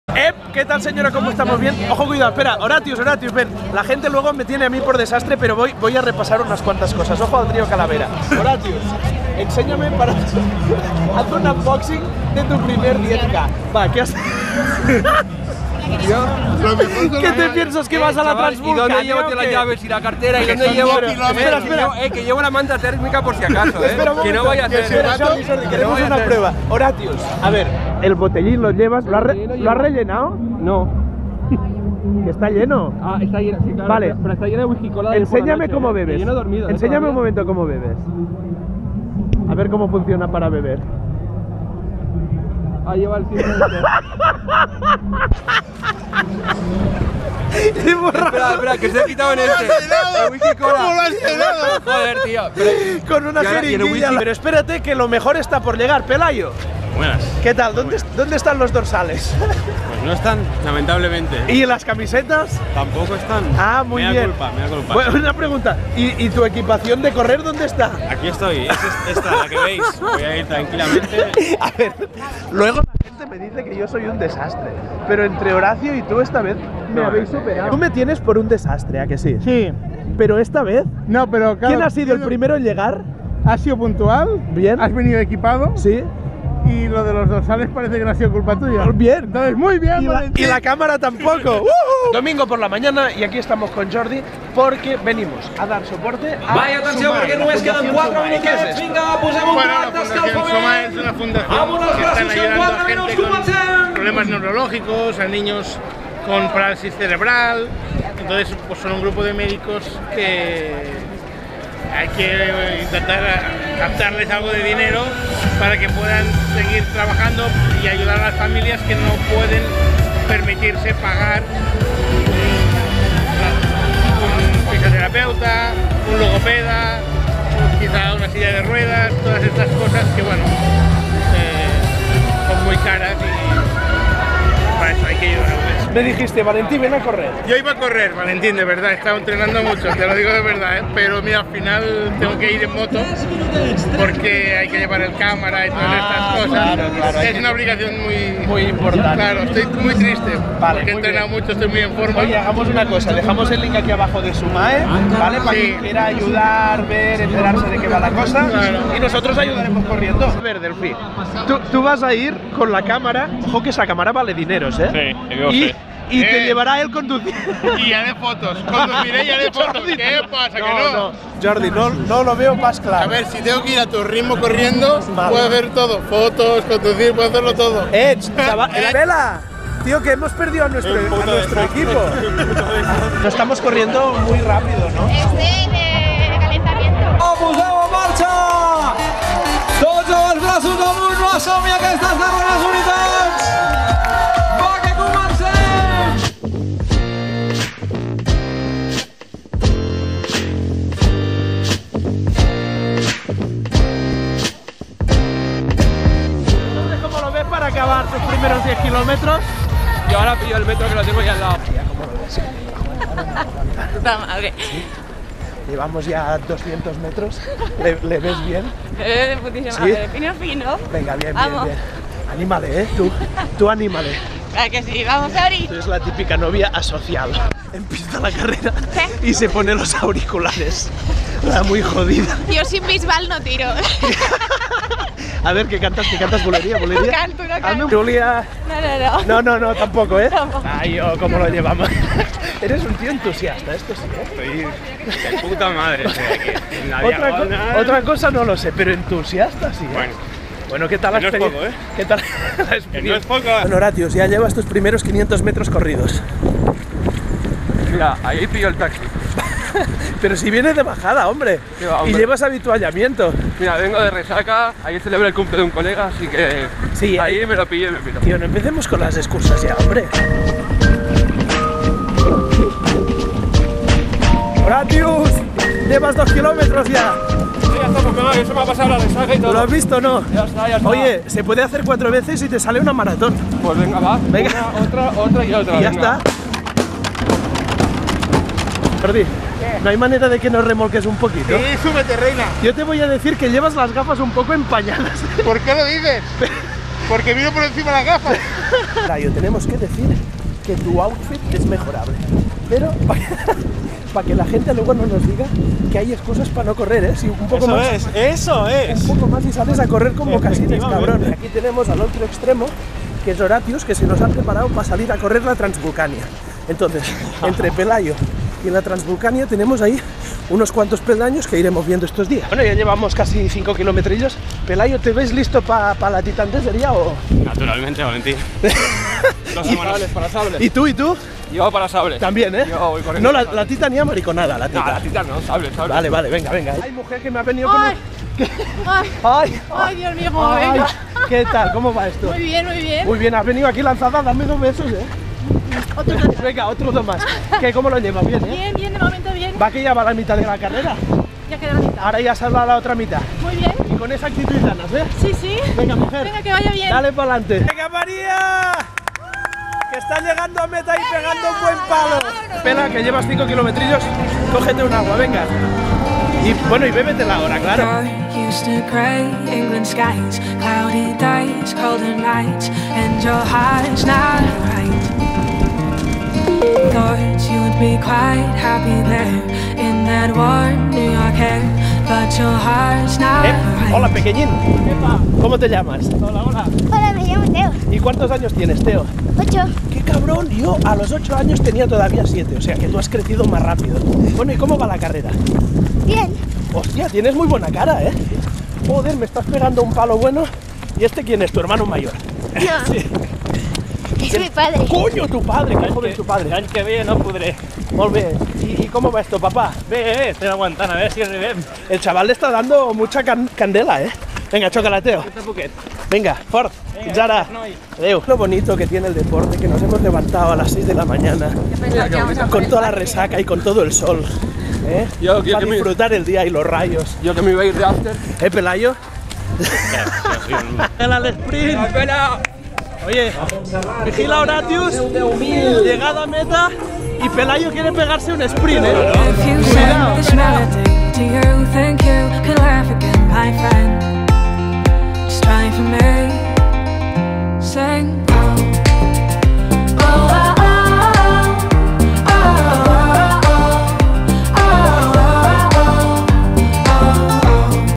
La Primera Carrera De Horatius
Aquí va la primera carrera de Horatius en familia: con Pelayo, María, Zai, Júlia, Jordi y conmigo. Para recaudar dinero para la Fundación Summae.